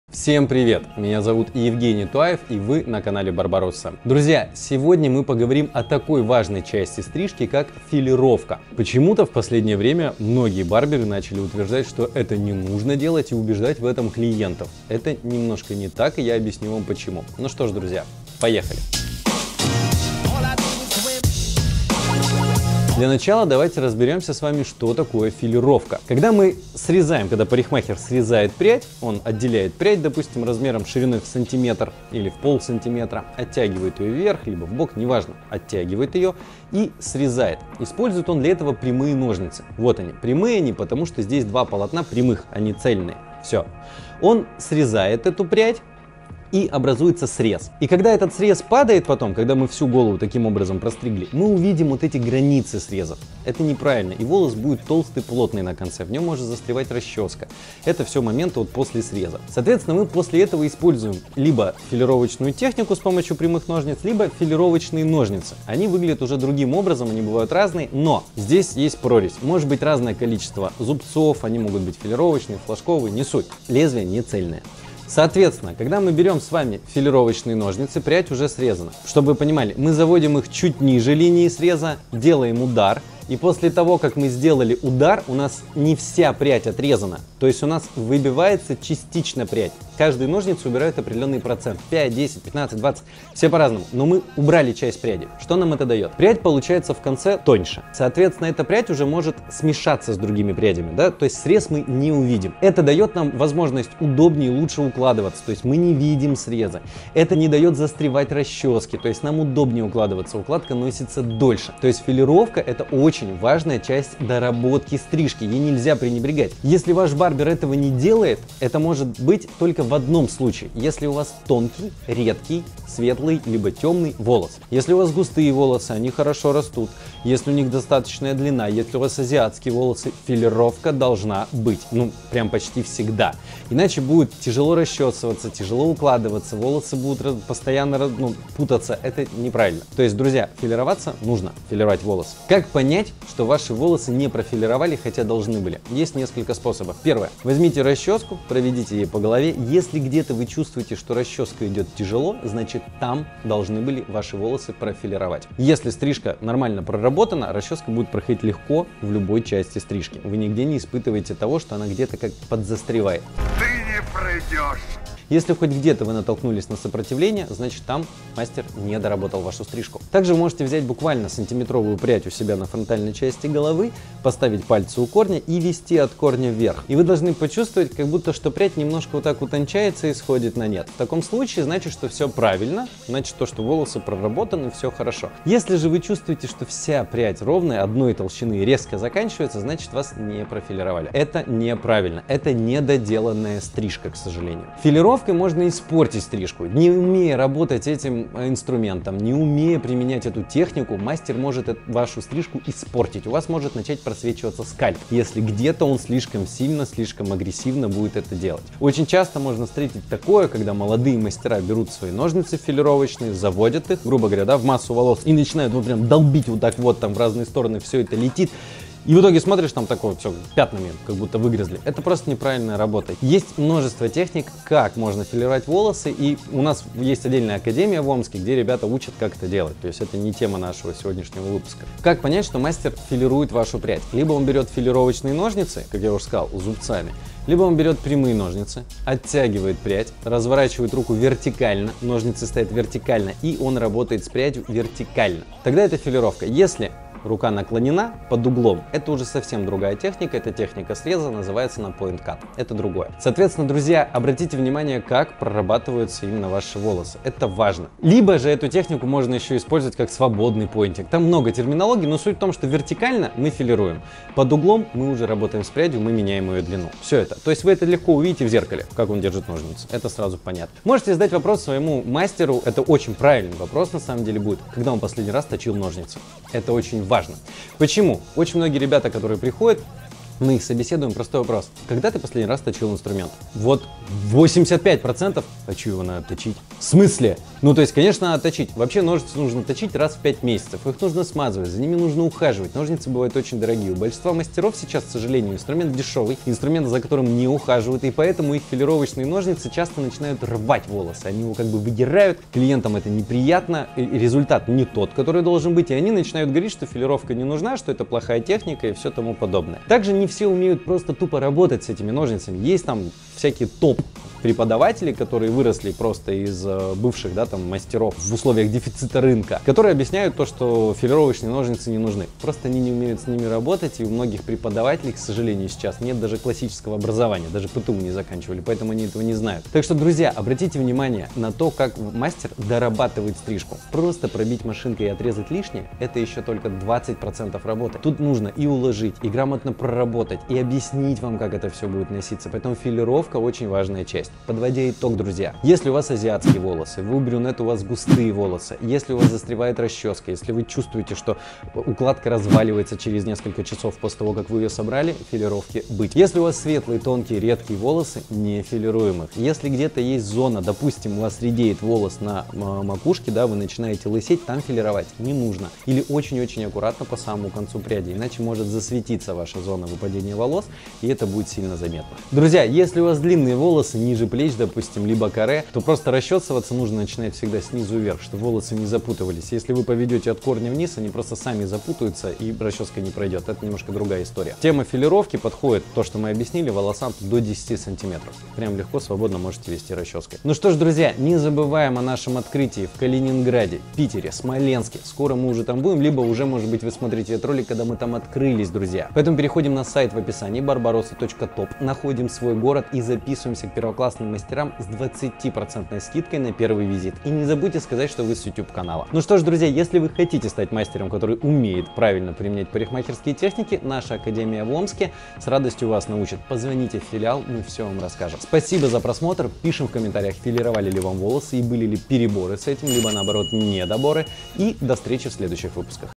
Что такое филировка волос? / Зачем делать филировку почти каждому клиенту?
Всем привет! С вами Евгений Туаев

Филировка волос - важная часть почти любой стрижки. Со временем многие барберы
начали убеждать клиентов в ненужности этого этапа. Я категорически с ними не согласен и
в этом выпуске объясню почему.

Сегодня расскажу, что такое филировка, зачем делать филировку почти каждому клиенту и
как понять, как филируют волосы.

0:00 - Филировка
0:23 - Что такое филировка?
2:22 - Как понять, что ваши волосы не профилировали?
3:14 - Как филировка может испортить стрижку?
3:52 - Как понять, что ваши волосы филируют?
4:31 - Вопрос мастеру «Когда точили в последний раз ножницы?»
5:44 - Итоги видео
6:30 - Заключение

🎦 Для коммерческих запросов писать на почту